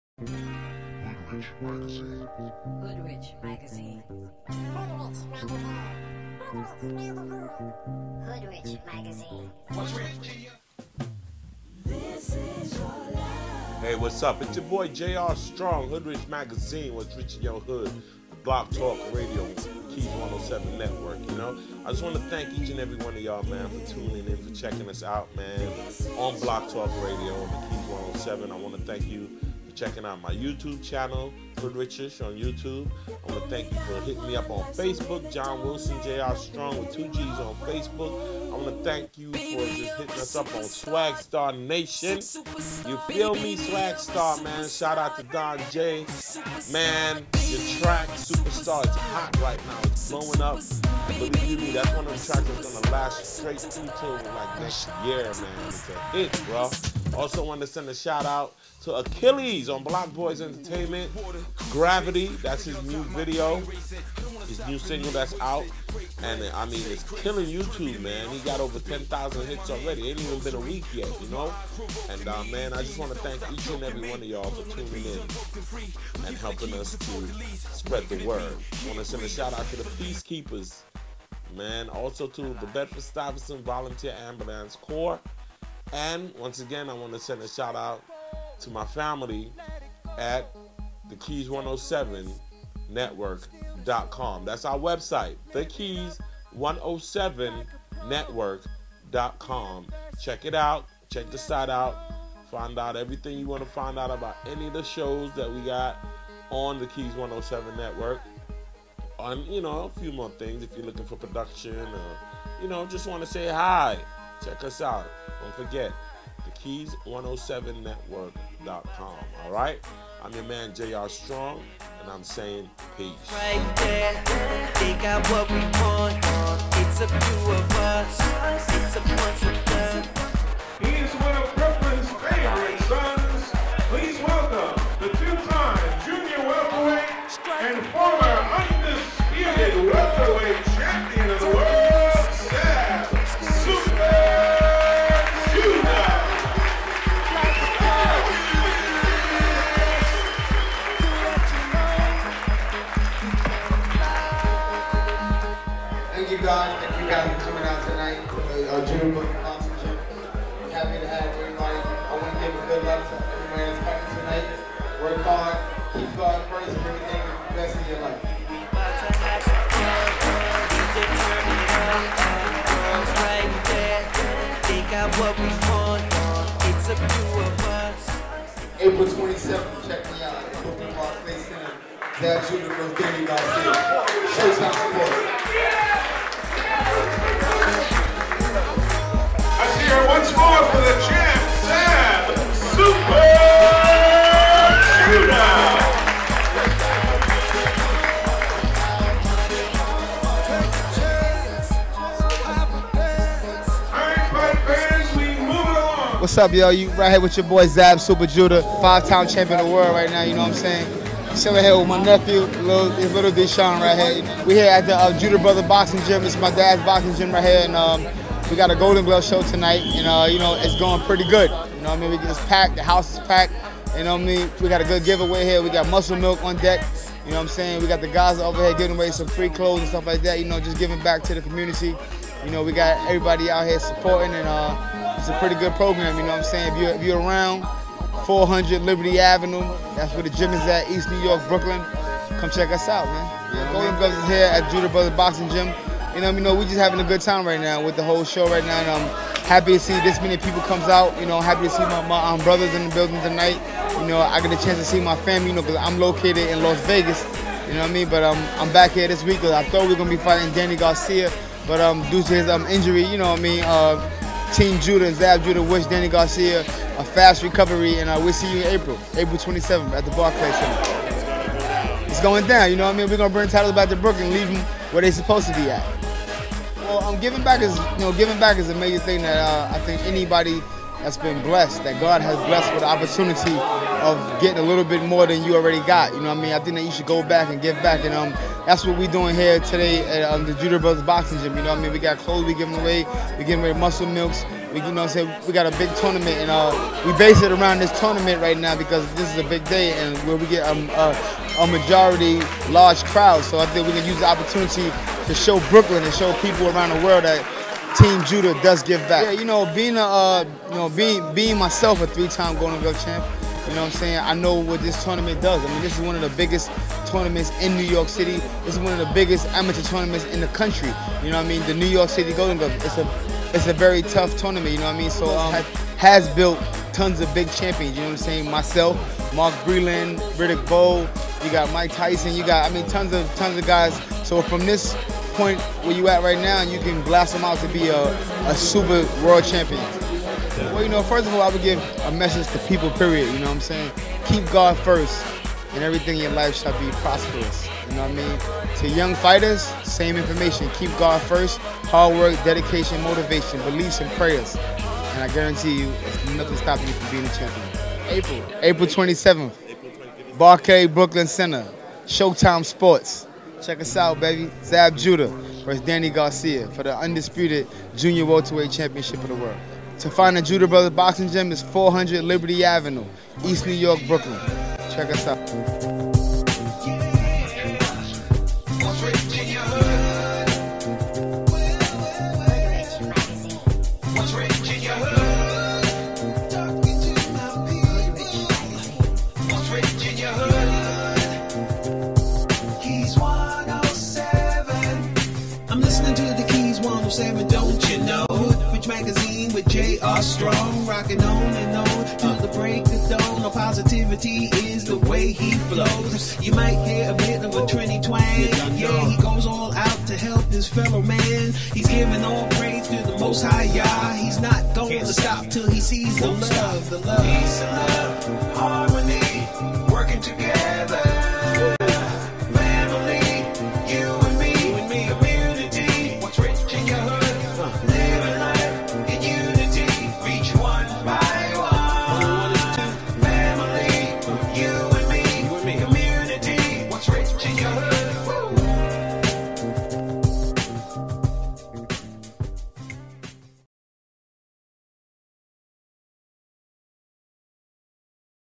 HOODRICH MAGAZINE TV EPISODE1: ZAB JUDAH @Judah Brothers Gym Brooklyn Golden Gloves Competition
J.R.Strong interview with 5 times world champion boxer Zab Judah @ the golden Gloves competition in Brooklyn NY The Judah Brothers Gym 400 Liberty Avenue East New York.